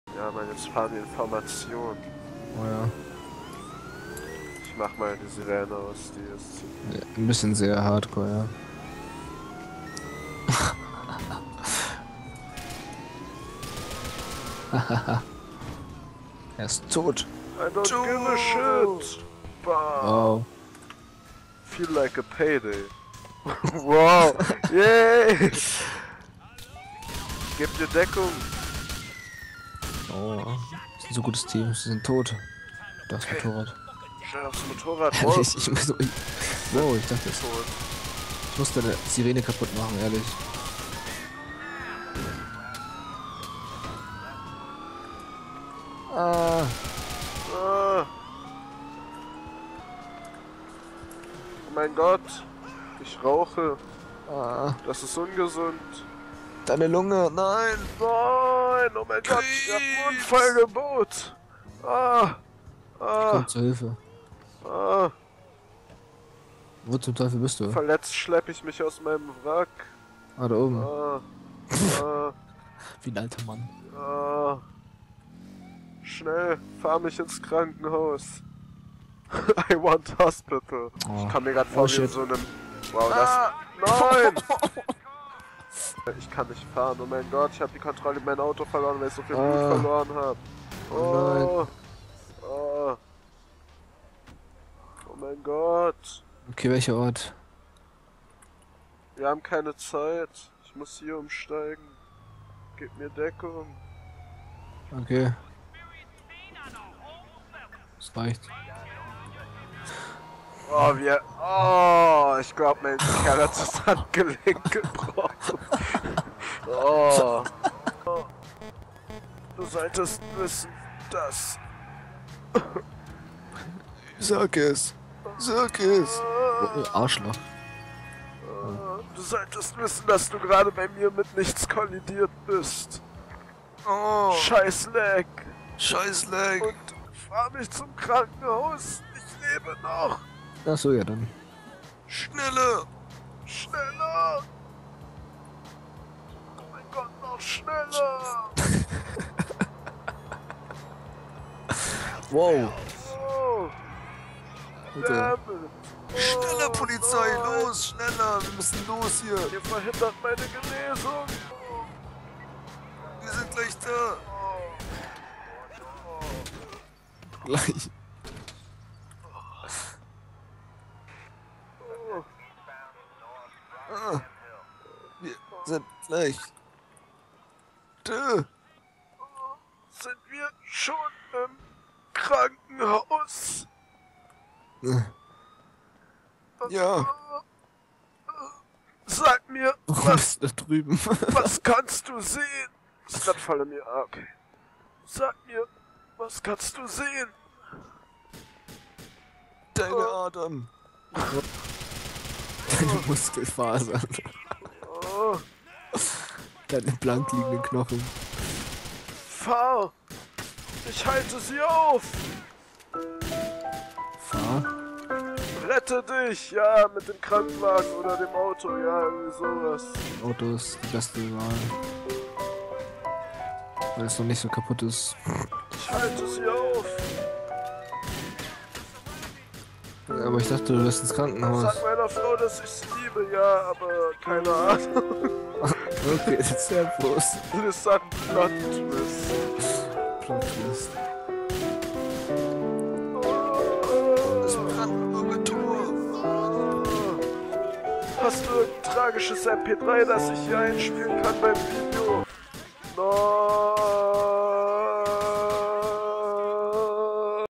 Killerlamm will Krankenhaus
Es scheint alles so gut zu laufen, als ein Autounfall für immer alles verändert...